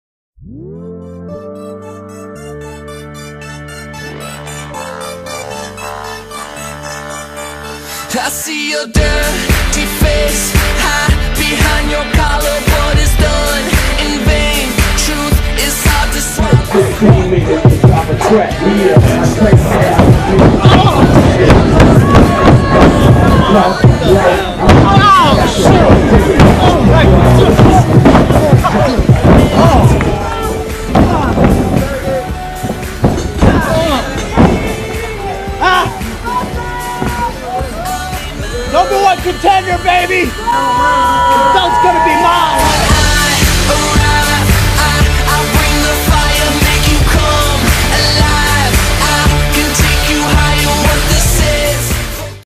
A Chris Evans Moment!
Here is a special message from Chris Evans himself. Will he win gold August 15th?